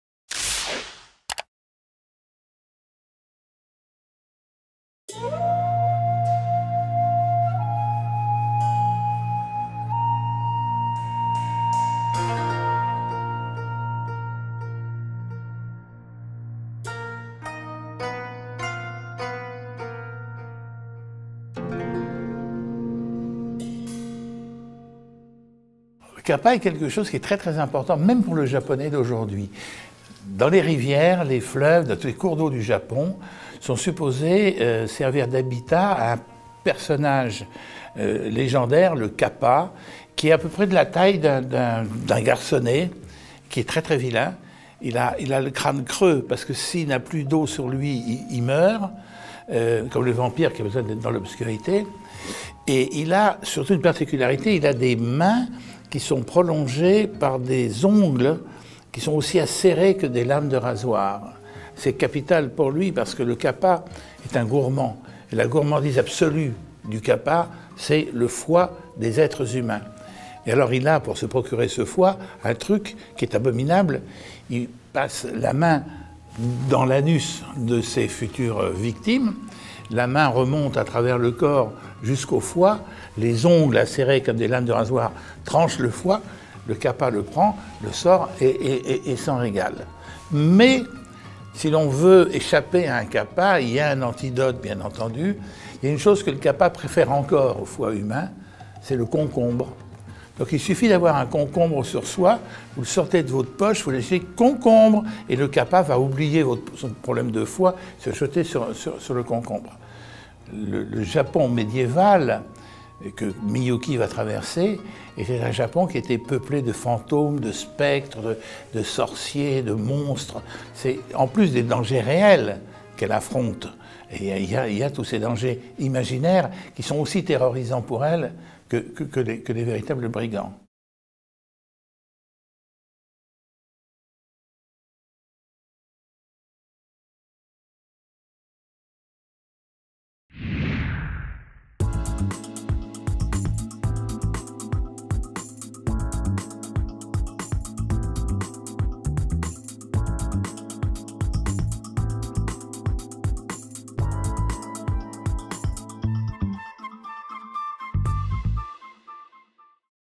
Le légendaire kappa japonais, par Didier Decoin
Didier Decoin dédicacera son dernier livre au Salon Histoire de Lire 2017 : "Le Bureau des Jardins et des Étangs" publié chez Stock. Dans cette histoire qui se déroule au XIIème siècle, le romancier fait subir mille tourments et péripéties à son héroïne japonaise, dont : LA FUREUR DES KAPPAS. Un être malfaisant à découvrir dans cette vidéo.